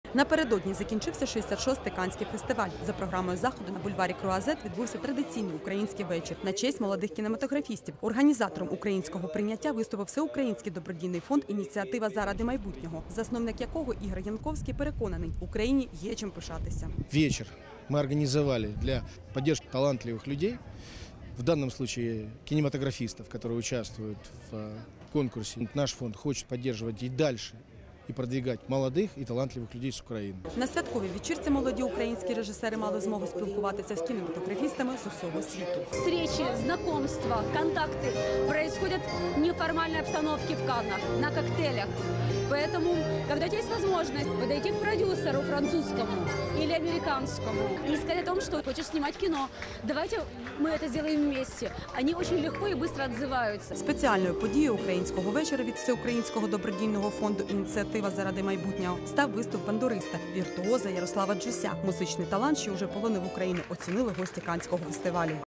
Фонд «Инициатива во имя будущего» провел прием на фестивале в Каннах
23 мая 2013 на бульваре Круазетт в Каннах (Франция) состоялся трехчасовой украинский прием в честь молодых кинематографистов, принимающих участие в работе Short Film Corner - официальной программе 66-го Каннского кинофестиваля, а также по случаю 25-летия присутствия Международного кинофестиваля «Молодость» в Каннах. Прием был организован фондом "Инициатива во имя будущего" (глава фонда - Игорь Янковский).

On Thursday, May 23 the Igor Iankovskyi National Charity Foundation "Initiative for the Future", headed by Igor Yankovskiy, held at the Boulevard de la Croisette in Cannes (France) the Great Ukrainian reception in honor of the young filmmakers involved in the Short Film Corner - the official program of the 66th Cannes Film Festival, as well as on the occasion of the 25th anniversary of the International Film Festival "Youth" presence in Cannes.